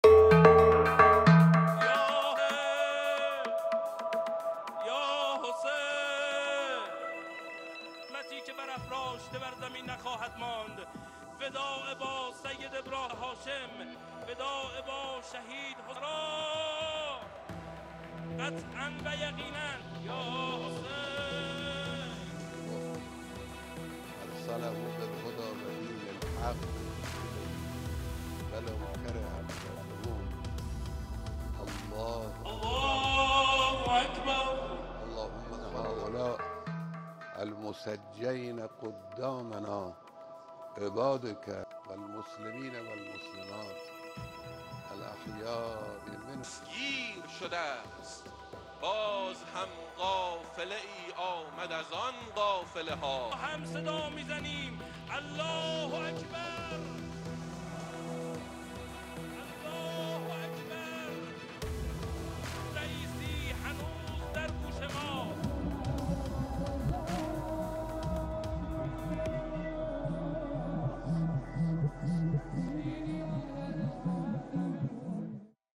Massaverdriet voor de verongelukte Iraanse president Raisi
Een indrukwekkende massa neemt in de Iraanse hoofdstad Teheran afscheid van hun president die verongelukte bij een helikoptercrash.